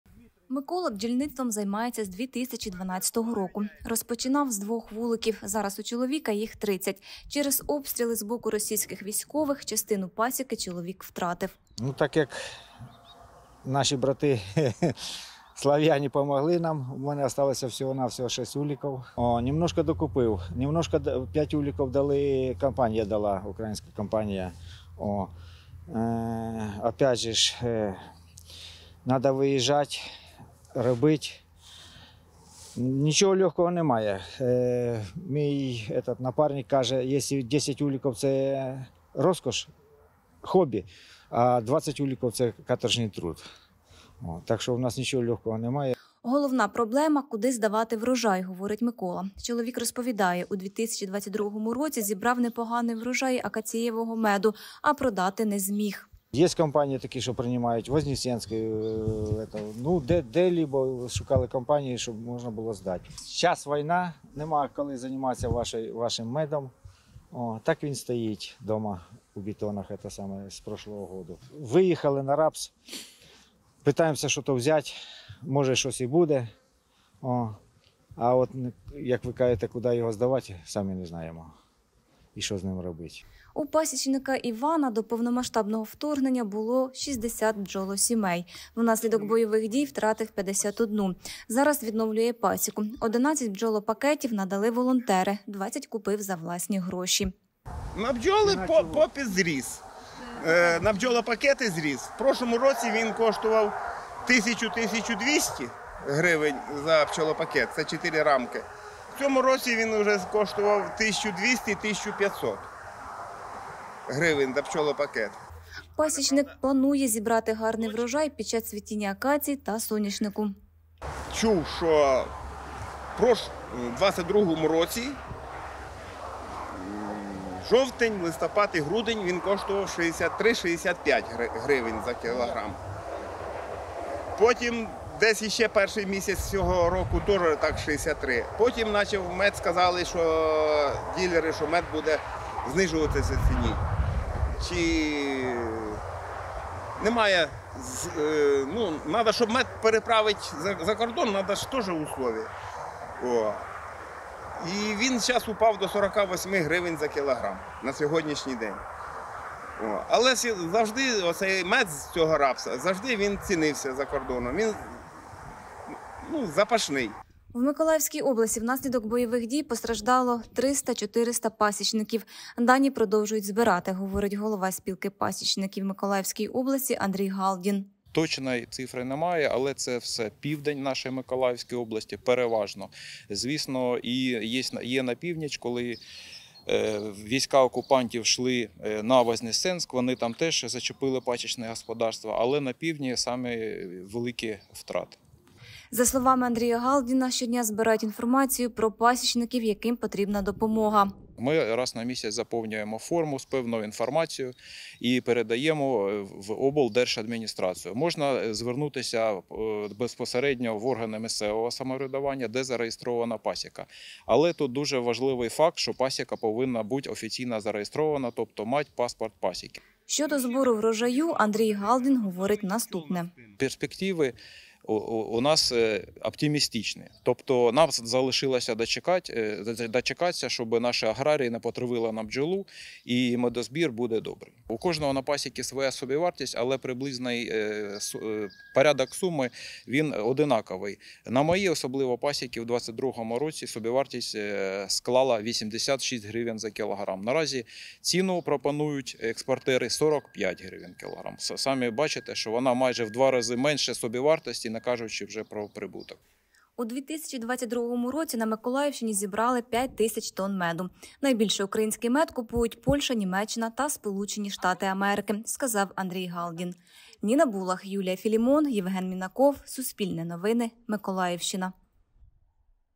На Миколаївщині від наслідків ведення бойових дій загинуло близько трьох тисяч сімей бджіл
В Миколаївській області внаслідок бойових дій постраждало до 400 пасічників, дані продовжують збирати. Станом на 15 травня зафіксована загибель близько трьох тисяч сімей бджіл, в одній в середньому налічується 20 000 тисяч комах. Переважно йдеться про південь області та Вознесенщину.